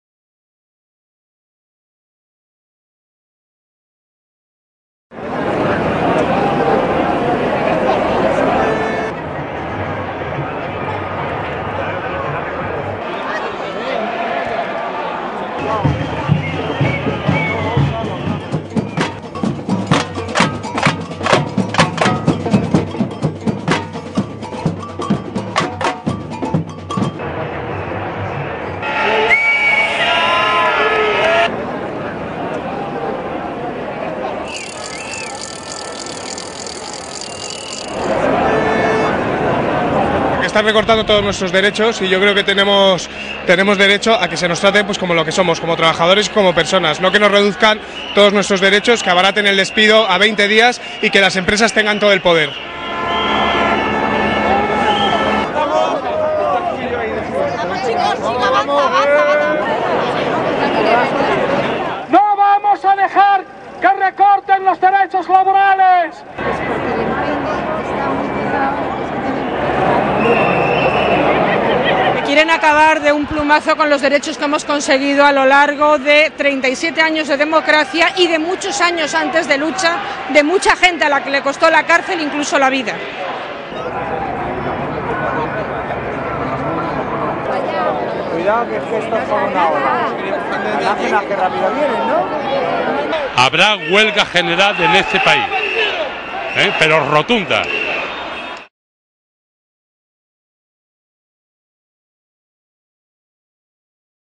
Masiva protesta de sindicatos en España contra ley de flexibilización laboral - 19-02-2012
Alrededor de un millón de personas protestaron el domingo en toda España contra las nuevas reformas laborales, las cuales fueron introducidas por el Gobierno del Partido Popular de Mariano Rajoy el 11 de febrero.
Los sindicatos de la España del trabajo organizaron manifestaciones en 57 lugares diferentes este domingo, donde los sindicatos de trabajadores estima que alrededor de 500.000 trabajadores participaron en la marcha
Las reformas pretenden reducir la indemnización a los que pierdan sus puestos de trabajo y permite a las empresas despedir a los trabajadores con una indemnización a 20 días  por año (antes era de 45 días), por ese motivo los trabajadores aseguran que si esta ley sigue adelante llegarán a la huelga general.
La ruta de la manifestación de Madrid pasó de la Plaza de Neptuno a la emblemática Puerta de Sol.